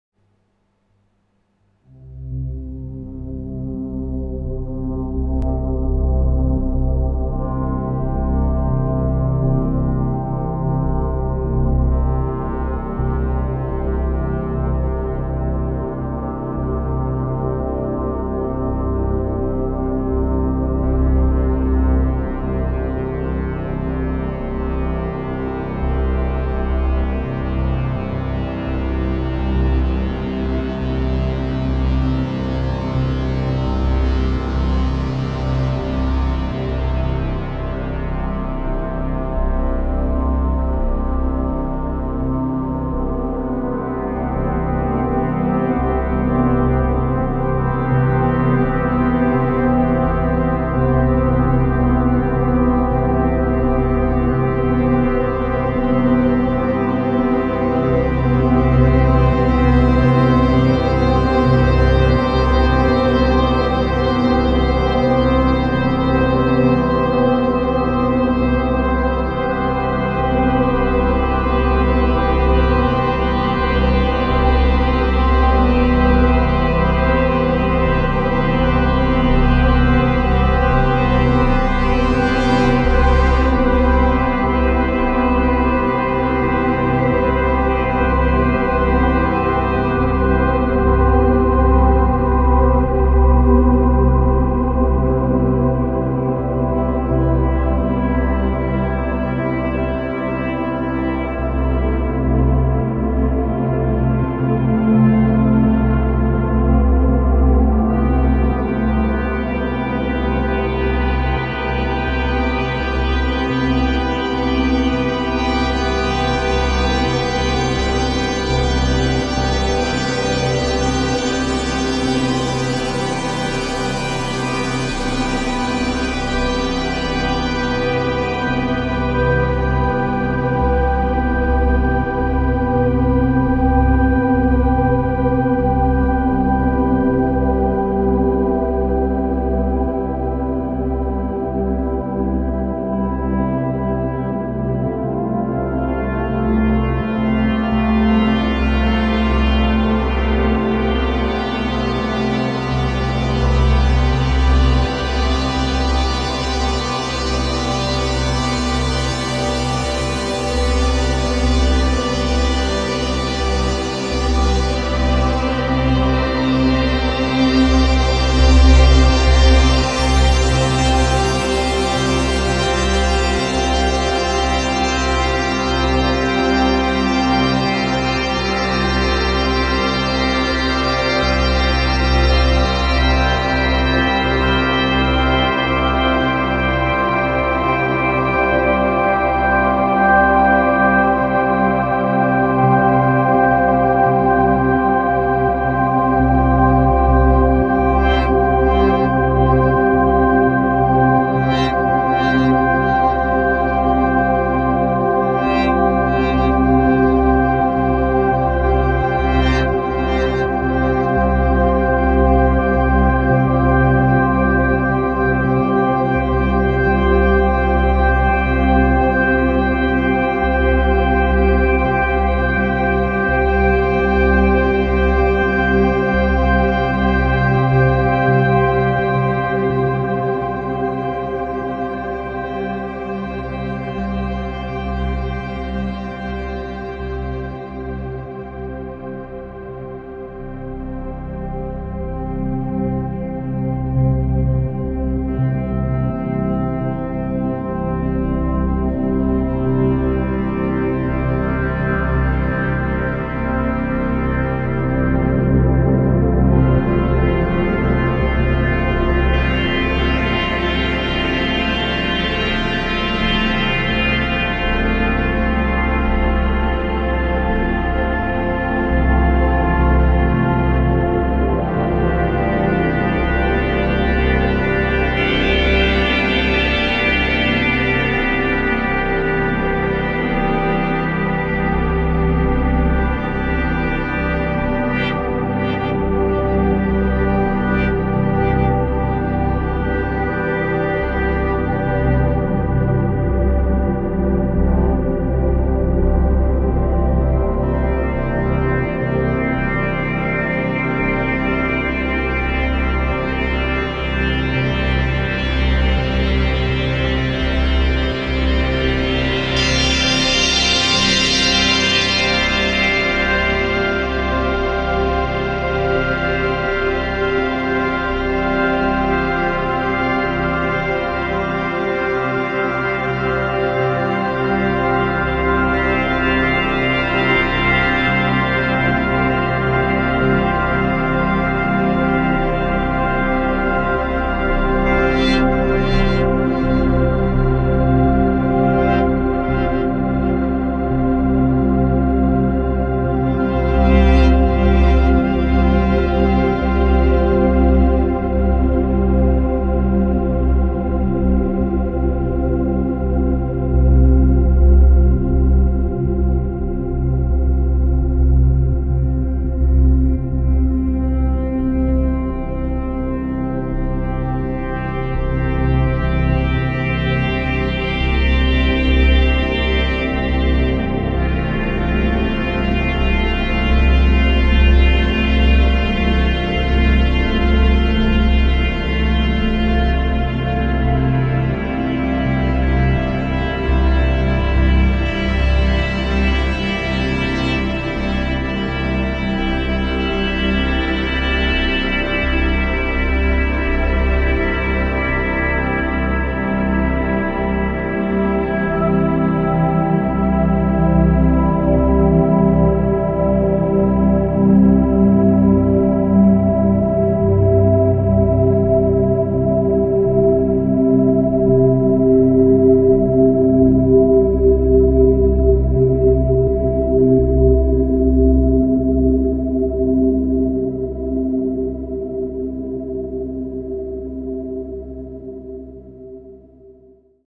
Dave Smith Prophet 12 - Strymon BigSky - Eventide TimeFaktor - Zoom G3X Drone Track
Just 3 awesome Stompers with a very dry P12 Pad awesome